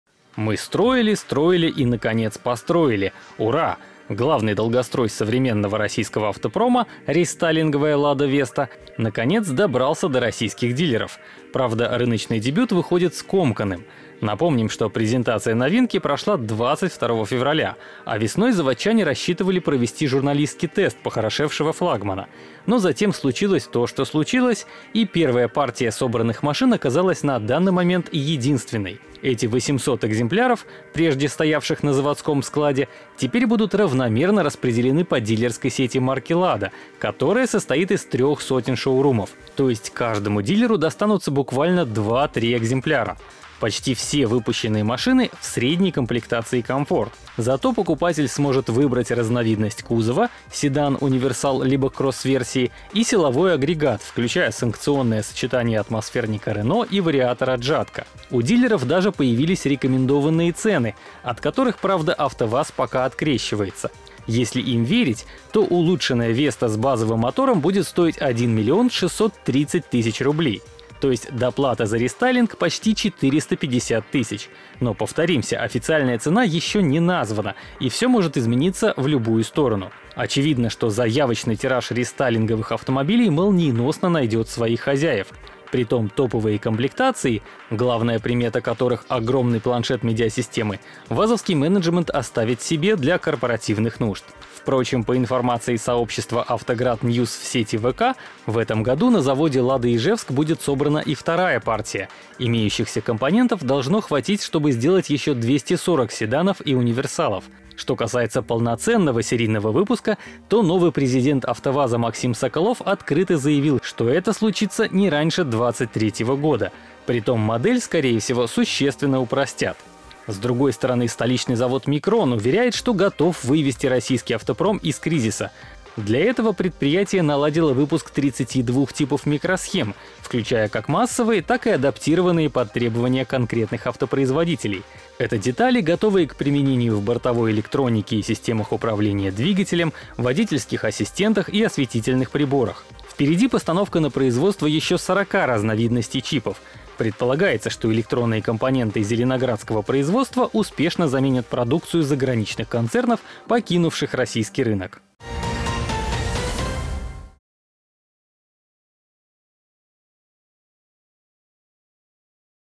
Lada Vesta попросит доплатить за рестайлинг. Завод «Микрон» обеспечит микрочипами | Новости №2121
Мы строили-строили, и, наконец, построили. Ура! Главный долгострой современного российского автопрома, рестайлинговая Lada Vesta, наконец, добрался до российских дилеров. Правда, рыночный дебют выходит скомканным… Напомним, что презентация новинки прошла 22 февраля, а весной заводчане рассчитывали провести журналистский тест похорошевшего флагмана. Но затем случилось то, что случилось. И первая партия собранных машин оказалась на данный момент единственной. Эти экземпляров машин, прежде стоявших на заводском складе, теперь будут равномерно распределены по дилерской сети марки Lada, которая состоит из трёх сотен шоурумов. То есть каждому дилеру достанутся буквально два-три экземпляра! Почти все выпущенные машины — в «средней» комплектации Comfort. Зато покупатель сможет выбрать разновидность кузова (седан, универсал, либо кросс-версии) и силовой агрегат, включая «санкционное» сочетание атмосферника Renault и вариатора Jatco. У дилеров даже появились рекомендованные цены, от которых, правда, АвтоВАЗ пока открещивается. Если им верить, то улучшенная Веста с базовым мотором будет стоить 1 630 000 рублей. То есть доплата за рестайлинг — почти 450 тысяч. Но, повторимся, официальная цена ещё не названа, и всё может измениться в любую сторону. Очевидно, что заявочный тираж рестайлинговых автомобилей молниеносно найдёт своих хозяев. Притом топовые комплектации, главная примета которых — огромный планшет медиасистемы, вазовский менеджмент оставит себе для корпоративных нужд. Впрочем, по информации сообщества Avtograd News в сети VK, в этом году на заводе «Лада Ижевск» будет собрана и вторая партия. Имеющихся компонентов должно хватить, чтобы сделать ещё 240 седанов и универсалов. Что касается полноценного серийного выпуска, то новый президент АвтоВАЗа Максим Соколов открыто заявил, что это случится только не раньше 2023 года. Притом модель, скорее всего, существенно упростят... С другой стороны, столичный завод «Микрон» уверяет, что готов вывести российский автопром из кризиса. Для этого предприятие наладило выпуск 32 типов микросхем, включая как массовые, так и адаптированные под требования конкретных автопроизводителей. Это детали, готовые к применению в бортовой электронике и системах управления двигателем, водительских ассистентах и осветительных приборах. Впереди постановка на производство ещё сорока разновидностей чипов. Предполагается, что электронные компоненты зеленоградского производства успешно заменят продукцию заграничных концернов, покинувших российский рынок.